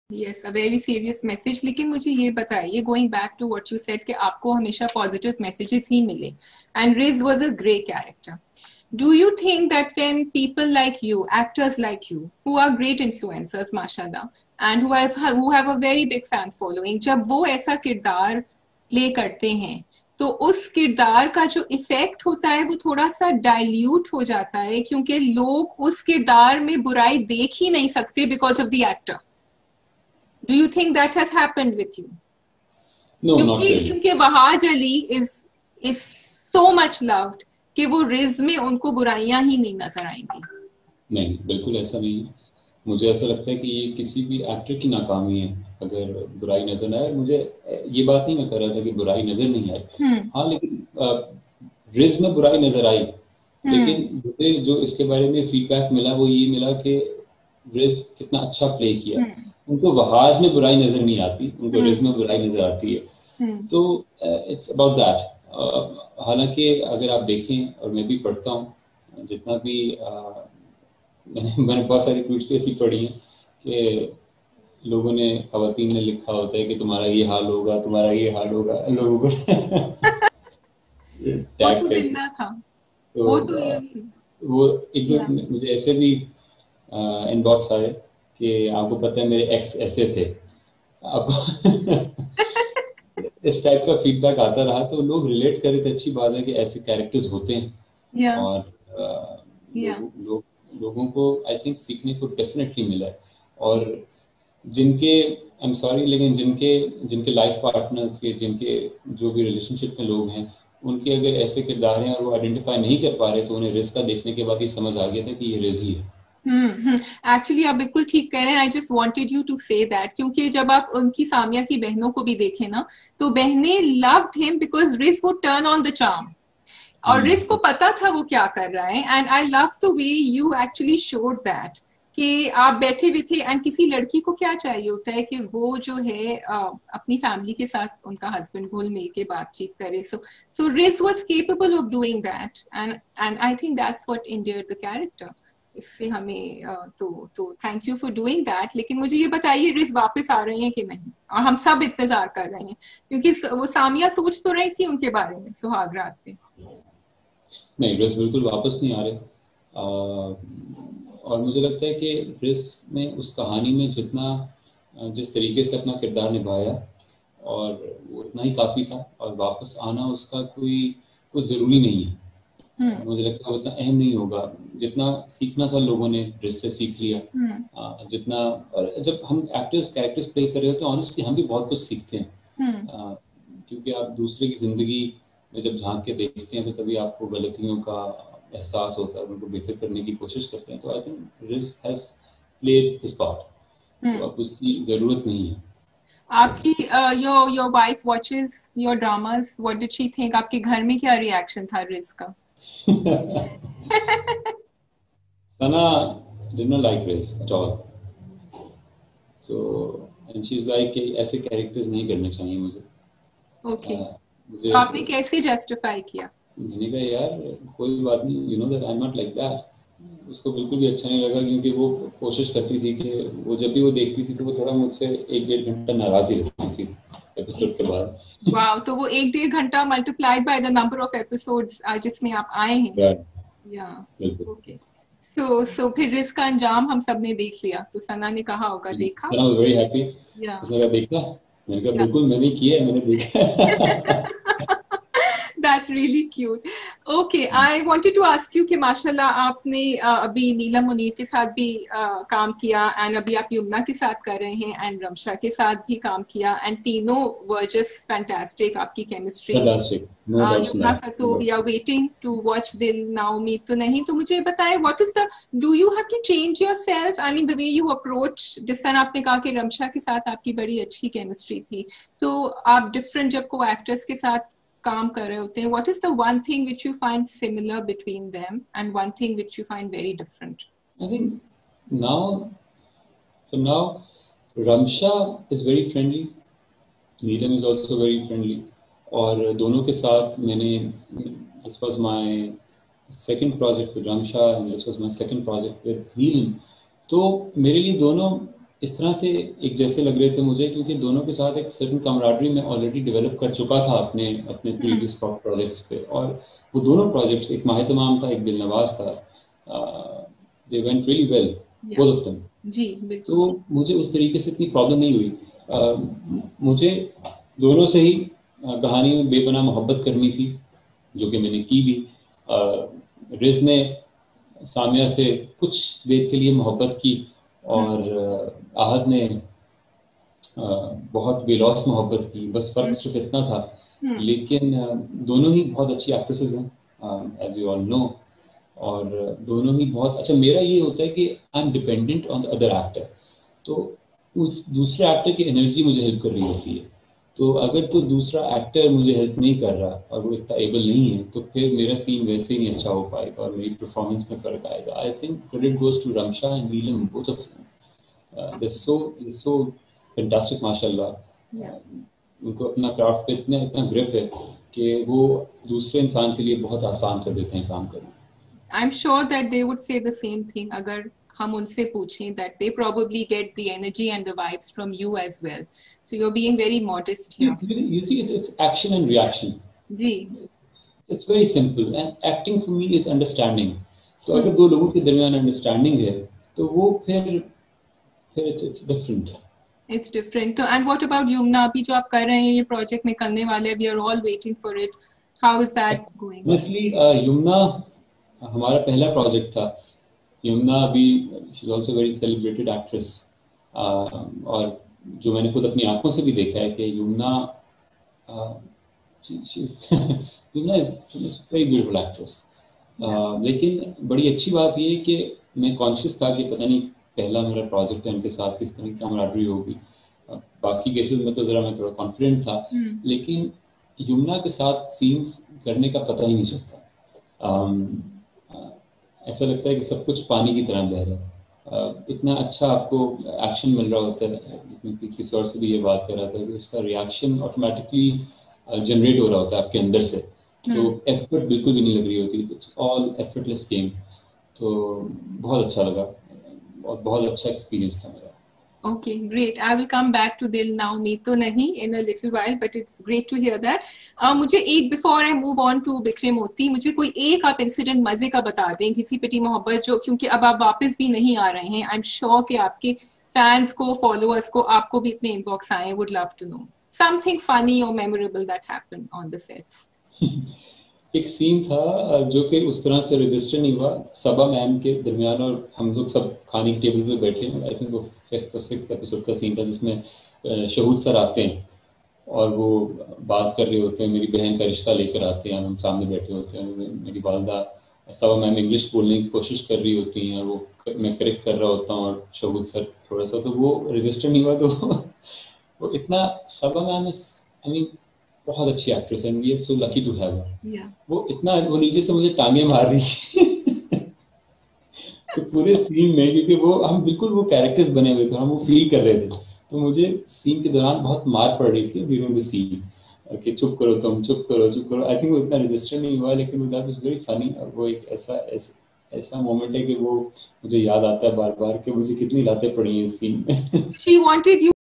Why Wahaj Ali's Wife Not Talking To Him? | Ghisi Piti Mohabbat | Wahaj Ali Interview | SB2Q
Why Wahaj Ali's Wife Not Talking To Him? | Ghisi Piti Mohabbat | Wahaj Ali Interview | Desi Tv | SB2Q